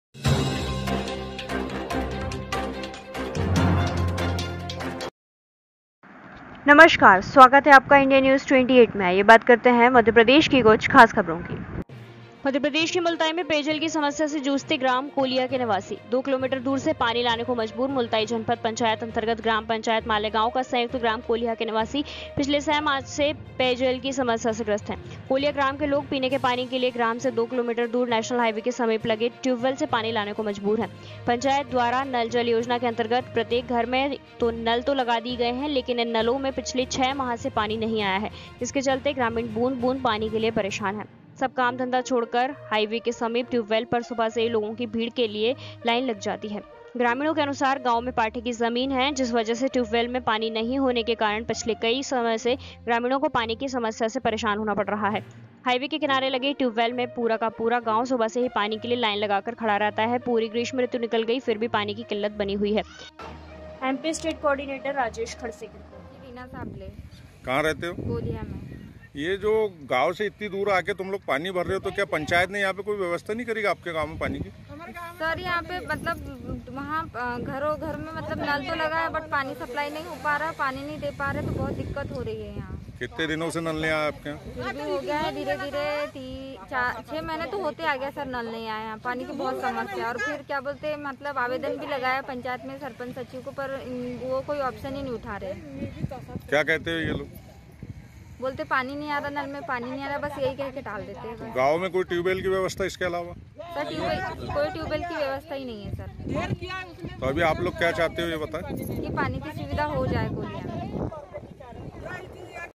बैतूल के मुल्ताई में पेयजल की समस्या से जूझते ग्राम कोल्हिया के निवासी
Official